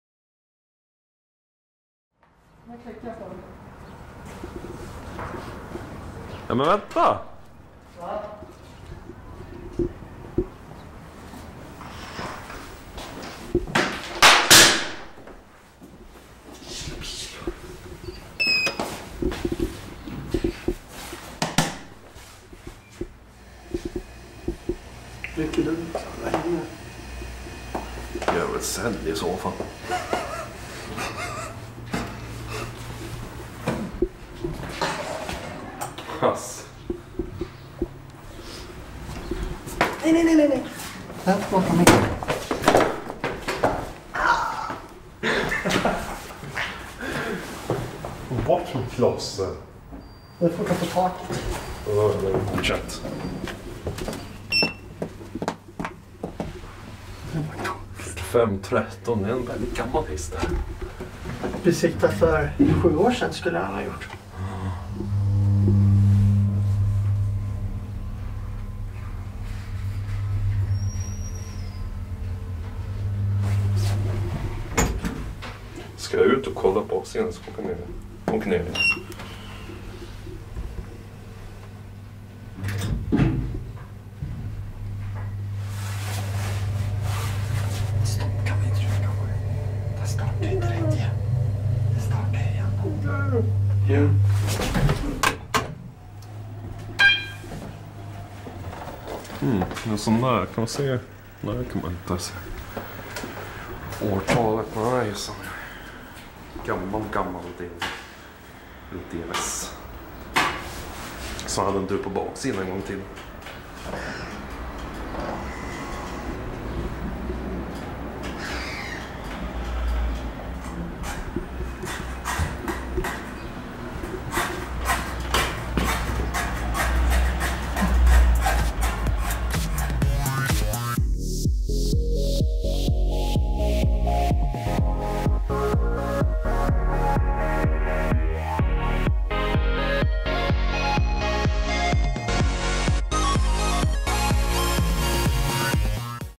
Old DEVE (MB: Schindler) Hydraulic elevator @ Willys Hagalund
ELEVATOR INFORMATION!

Manufacturer: DEVE
Installation year:
Current modernizer: Schindler
Latest modernization year:
Serial Number: K-4513
Capacity: 500Kg - 6 Persons
Floors: 2 = NED UPP

The elevator got new beeing buttons and full-height photocells on one side, while the other side was boarded up. Before

Location: WILLYS Hagalundshallen Grocery store - Hagalundsgatan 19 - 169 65 Solna (Hagalund/"Blåkulla") SWEDEN!